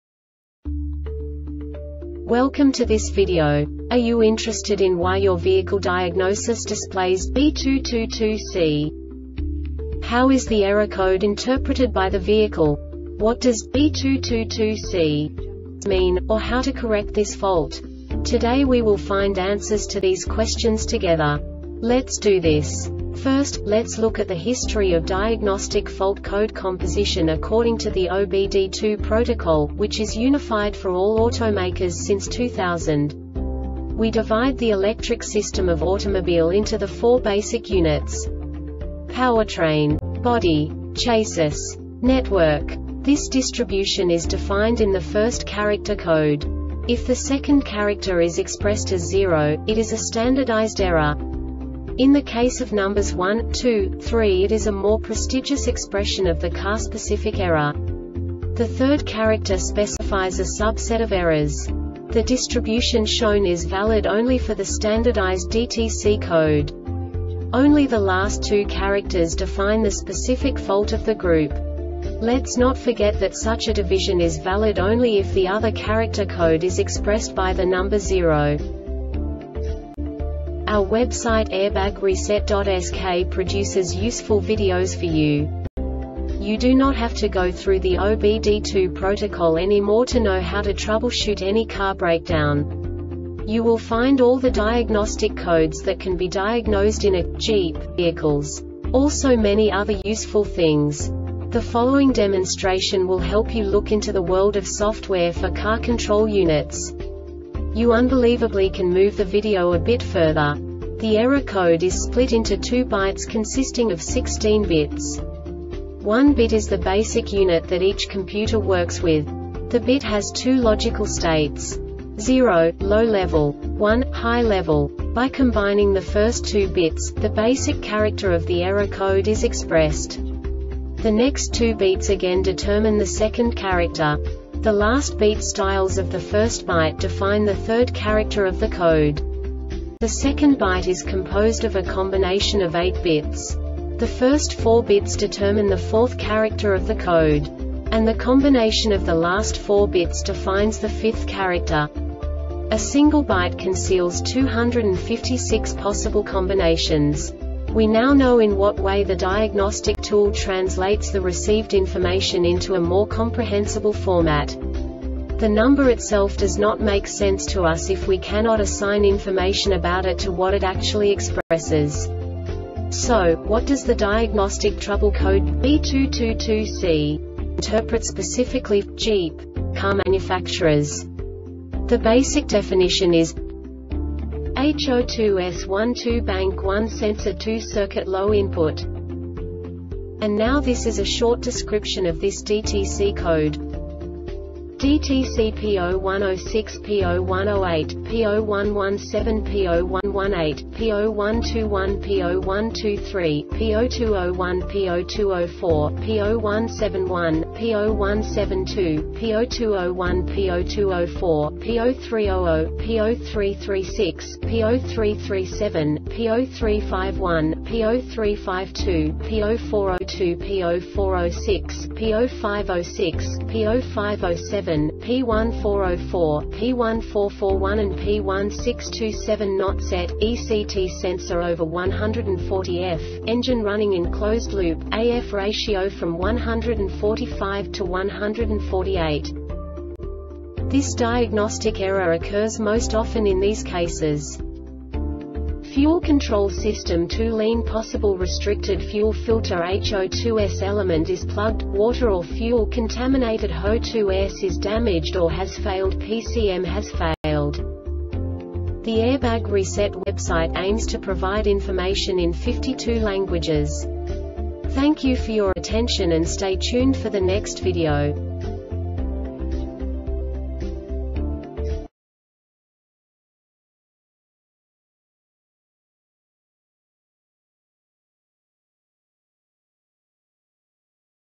DTC Jeep B222C Short Explanation
Contents:
0:21 Basic DTC analysis according to OBD2 protocol standard.
1:48 Insight into programming
2:58 A diagnostically understandable description of the Diagnostic Trouble Code
3:05 Basic error definition

Make:
Jeep
Code:
B222C
Definition:
HO2S-12 (Bank 1 Sensor 2) Circuit Low Input
Description:
Cause:
Fuel control system too lean (possible restricted fuel filter)HO2S element is plugged,water or fuel contaminatedHO2S is damaged or has failedPCM has failed